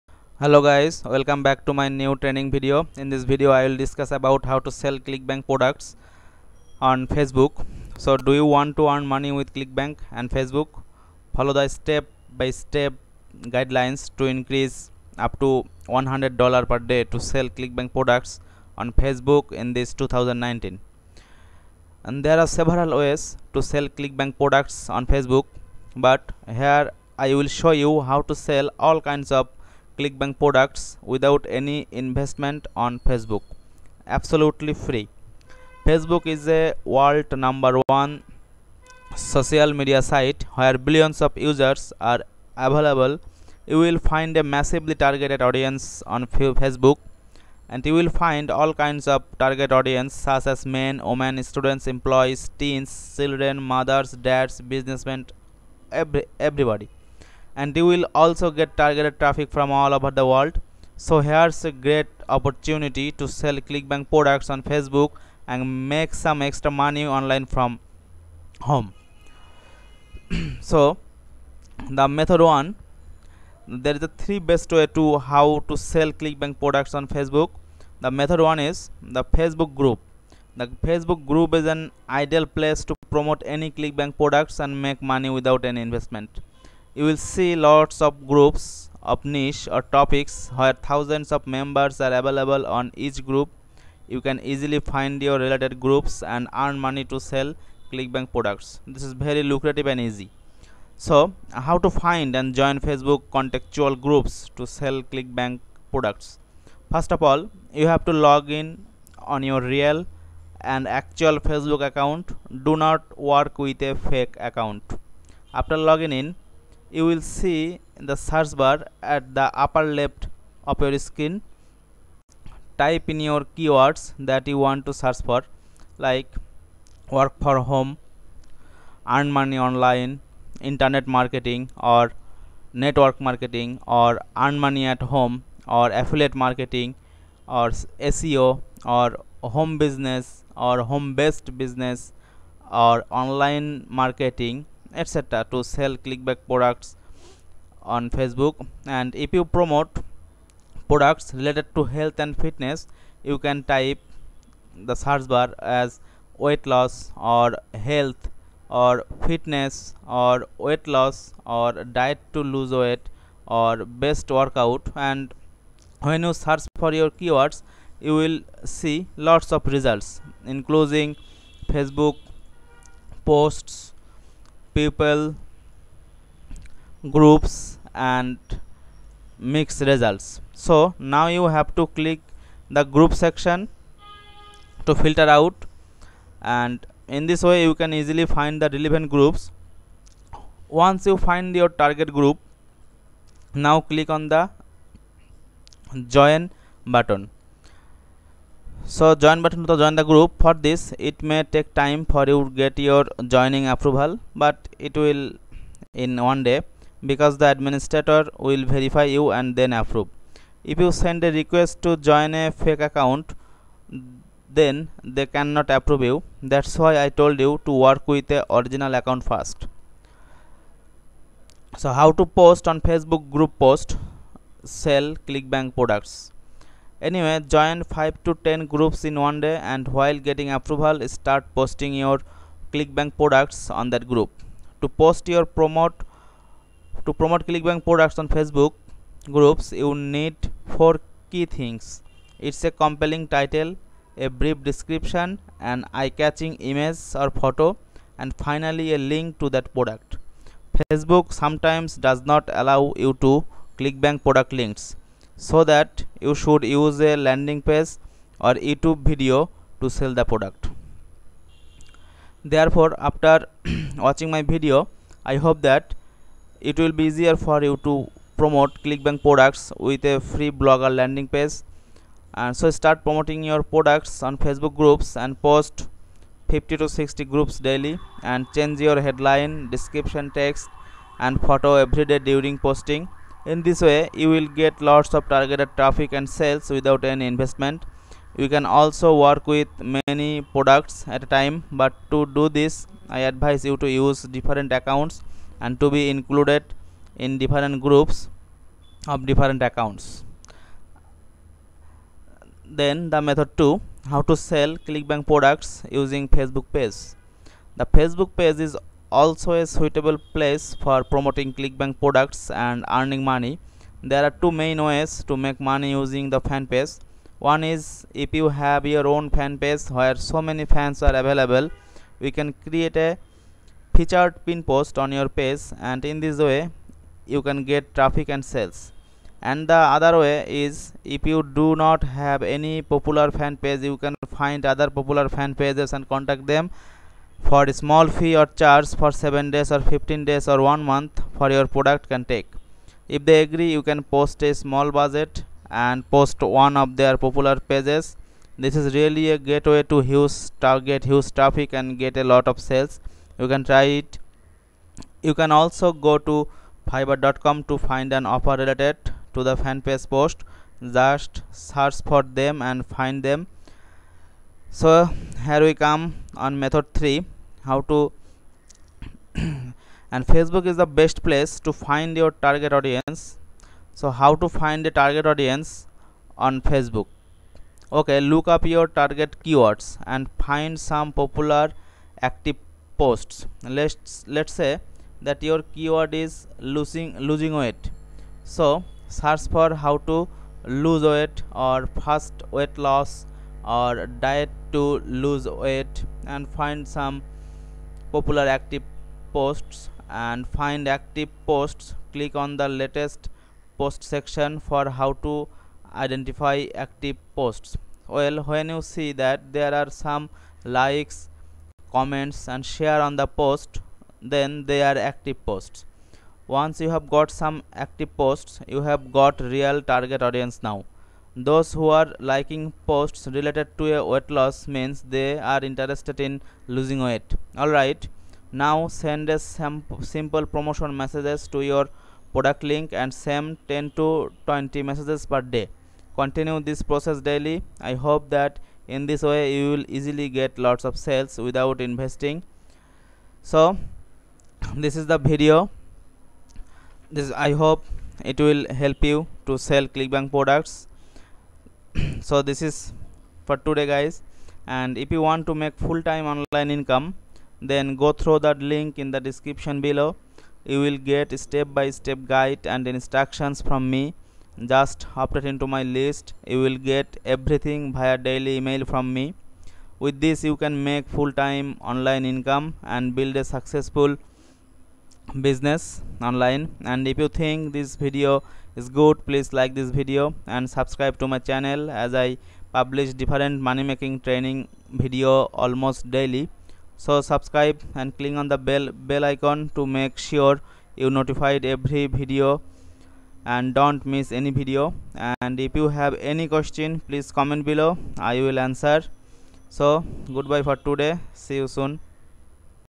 How To Promote Clickbank Products On Facebook in 2020 🔥 FREE 🔥
In this video I have shown how to promote clickbank product on facebook groups for free.

It works in 2020 and so on.

There are lots of facebook groups for every niche and category.

And there are lots of products for every niche in clickbank.

So, to promote clickbank products on facebook is so easy, you just need find your relevant groups and promote the clickbank products.

Hope this video will help you.